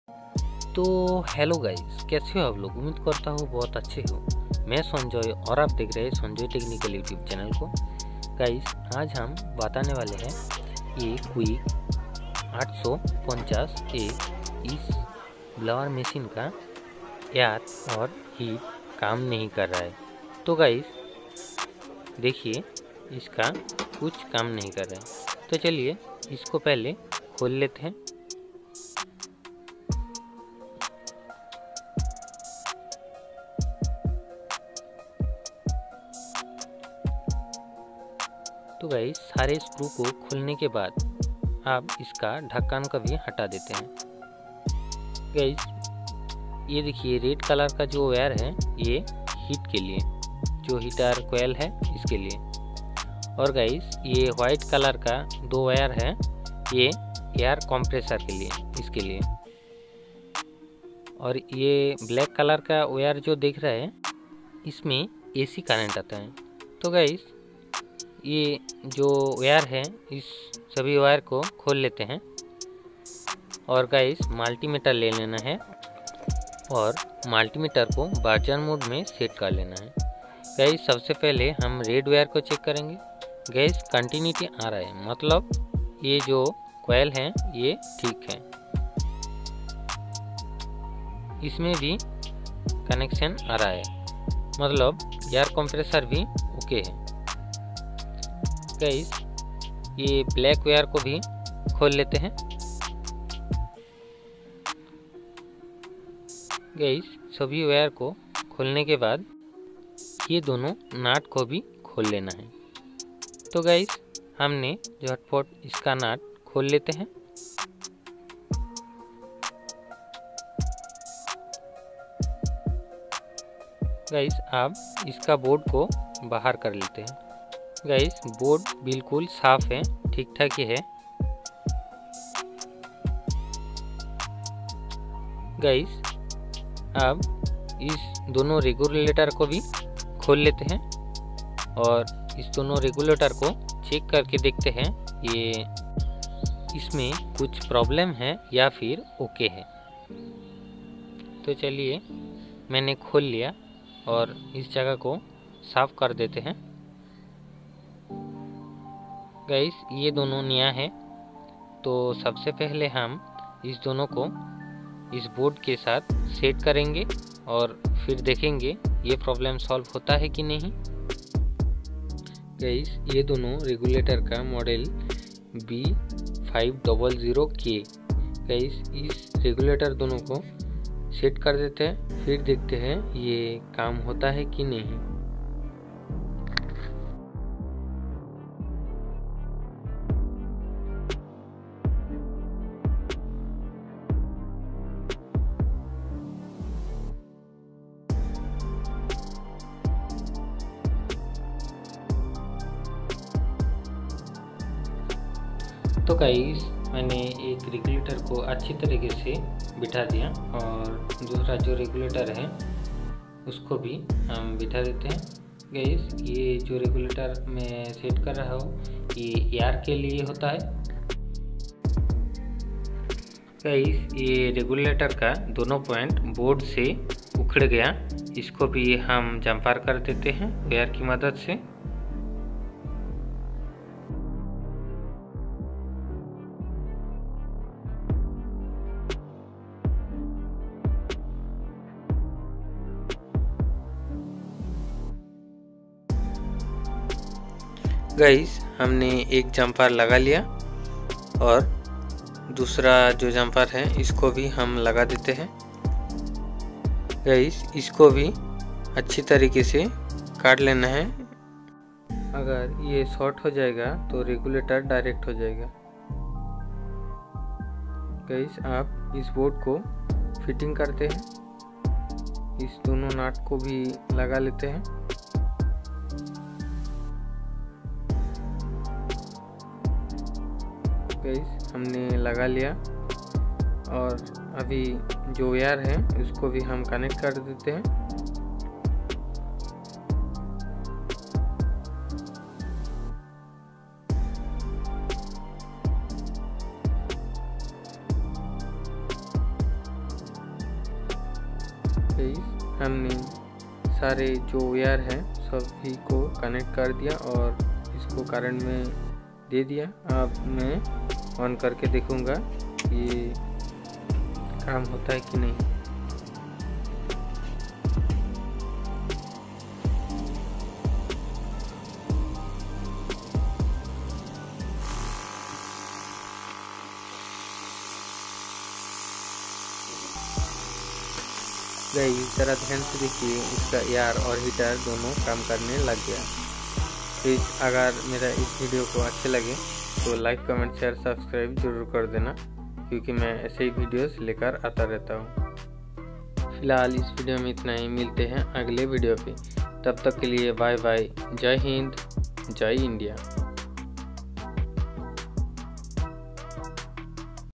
Quick 850A air and heat not working / smd rework station heat and air problem solve
Hey guys
        I am Sanjay Das

Video shoot by---BIJOY DAS

quick 850a air and heating problem
blower machine air and heating problem
smd rework station air and heating problem